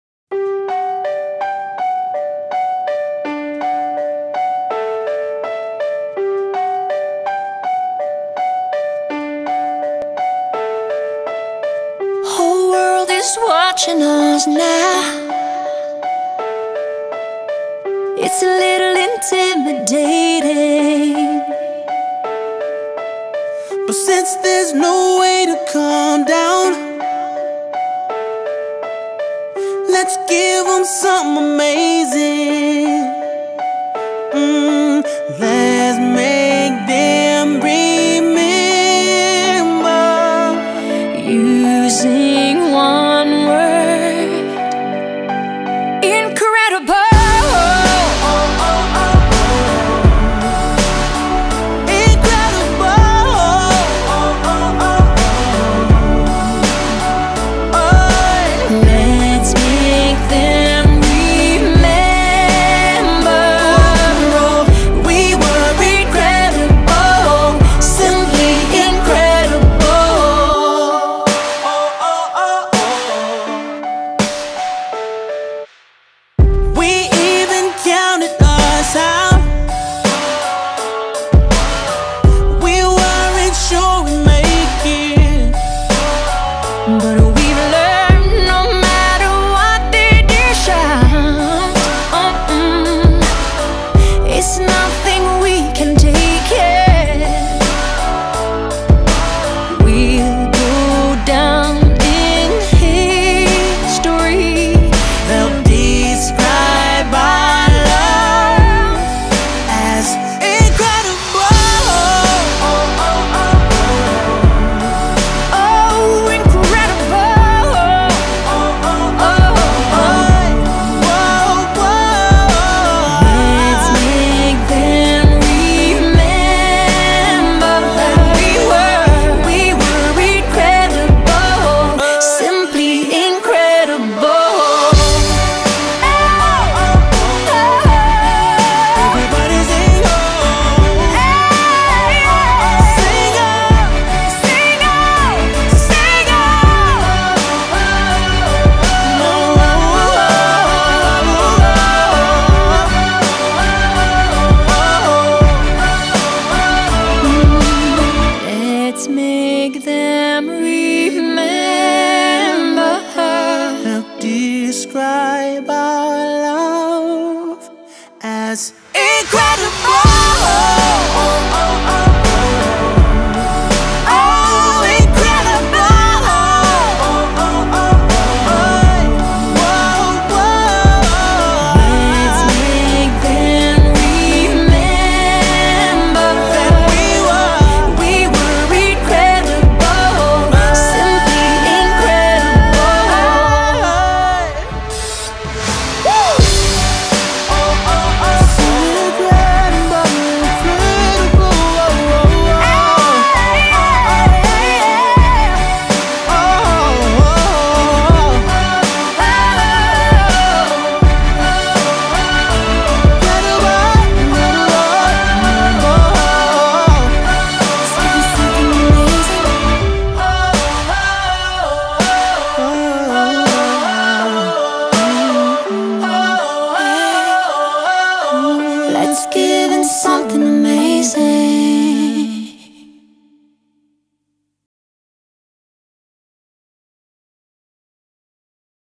Katia and Sergei15 Incredible Olympic version
A tribute to Pairs skaters GG and their two olympic golds.
19 years- missing Sergei.